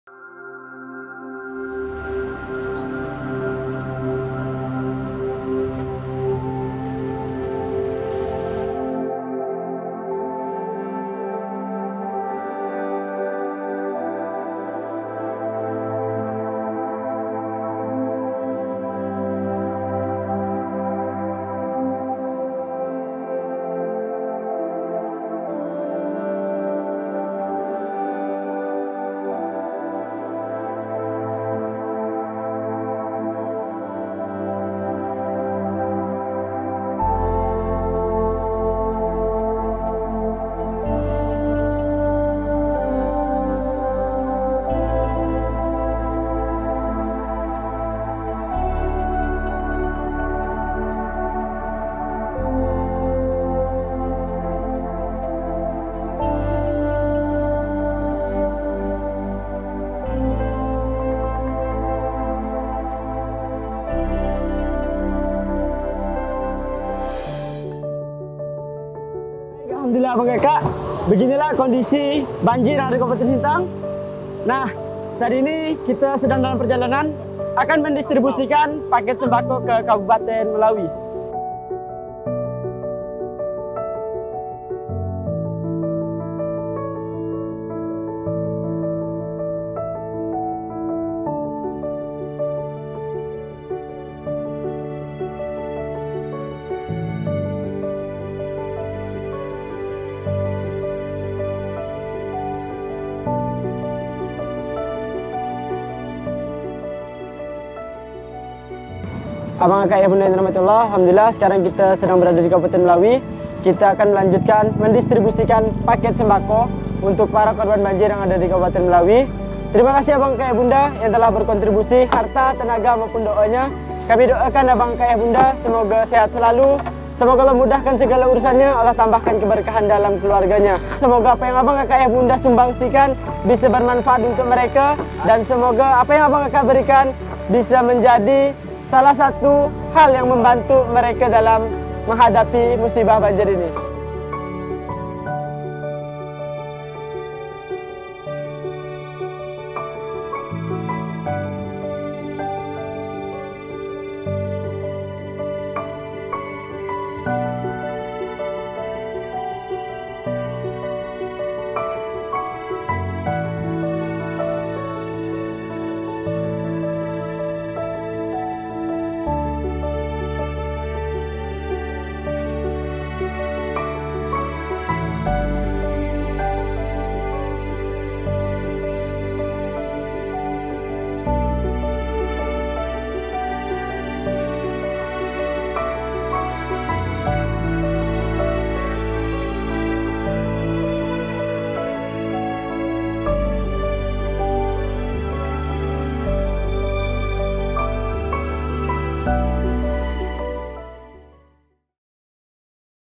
RIBUAN PAKET BANTUAN TELAH DIDISTRIBUSIKAN
A'udzubillahiminasyathoonirrojim
Bismillahirrahmanirrahim

Menurut update terbaru, kondisi di wilayah Sintang dan sekitarnya masih terendam banjir setinggi 3 meter (11/11) lalu.
Hingga saat ini, tim Paskas Rescue terus bergerak membantu mendistribusikan sembako di 4 Kabupaten terdampak banjir di Kalimantan Barat yaitu Sanggau, Sekadau, Melawi, dan Sintang masih

Sebab, masih banyak masyarakat sekitar yang membutuhkan bantuan dan support untuk memenuhi kebutuhan hidup. Apakah kamu salah satunya yang akan membantu mereka?

Terimakasih banyak kami ucapkan kepada orang-orang baik yang telah berdonasi, semoga Allah berikan balasan yang terbaik, dan semoga banjir di Kalimantan Barat segera surut. Aamiin Allahumma Aamiin 🤲

Jazakumullah khairan katsiran, semoga Allah jadikan ayah bunda abang kakak sebagai insan yang senantiasa bermanfaat untuk ummat :)

Alamat Kantor BMI Babel :
Jln. Depati Amir (Jln. Mentok) No.15 Rt.03 Rw.01 Kel. Melintang Kec. Rangkui, Pangkalpinang